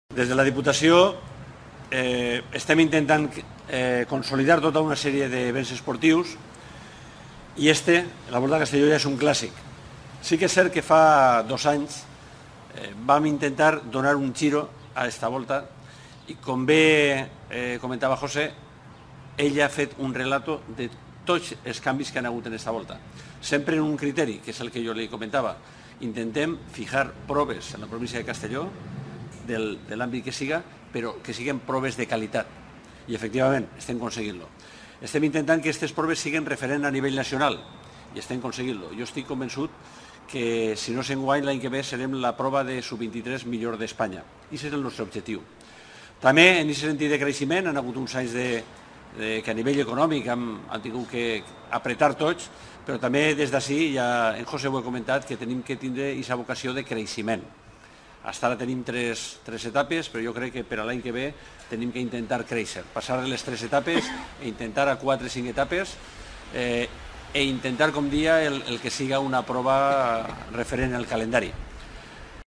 Presentación Volta Ciclista Castelló 2015
José Cabedo y Luis Martínez han presentado los detalles de la Volta a Castelló 2015 en la Diputación de Castellón. El director de la carrera y el diputado de deportes provincial han coincidido en destacar el buen trabajo de modernización de la carrera, pero también han reconocido que aún queda margen para hacer “mucho más ciclista nuestra provincia”.